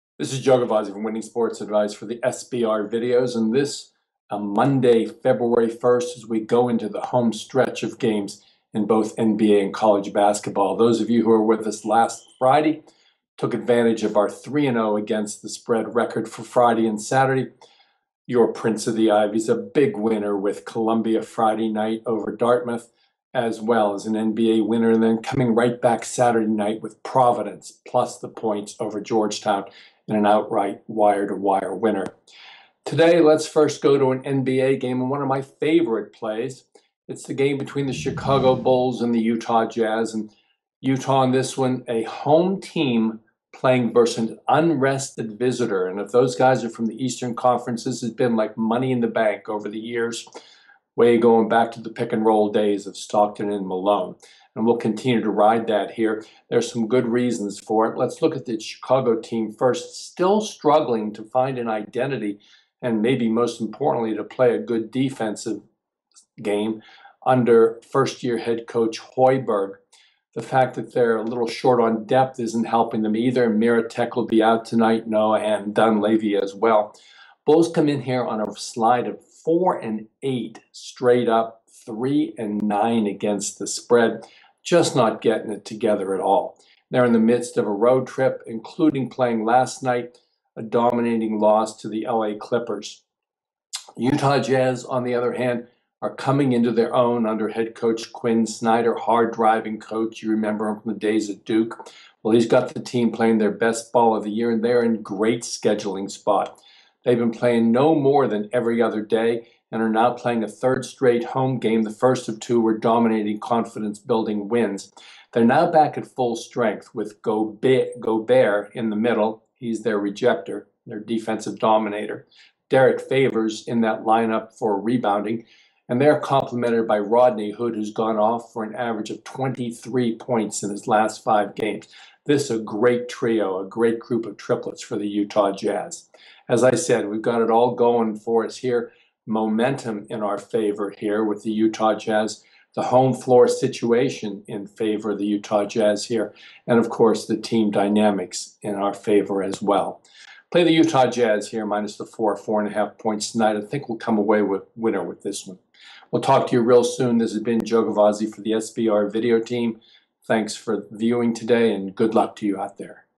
Chicago Bulls vs. Utah Jazz Best Betting Approach
Joe Gavazzi likes the home team Louisville  to cash this ticket. Join in, get the insight and information you need for your NBA Picks.